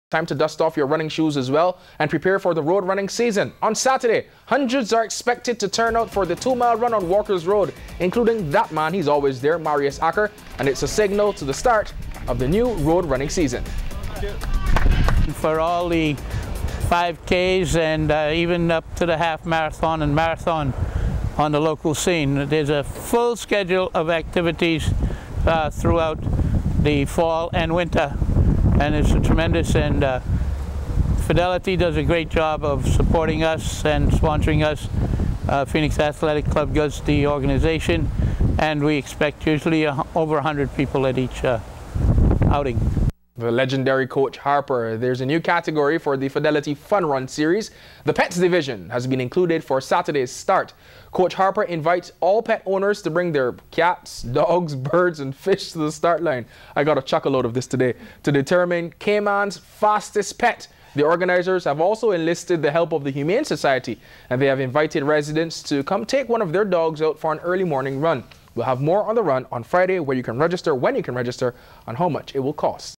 Fidelity Fun Run Series
Hundreds are expected to line up for Saturday's Fidelity Fun Run Series.

The three race series of two miles each signals the start of the road running season which includes the Cayman Islands Marathon in December.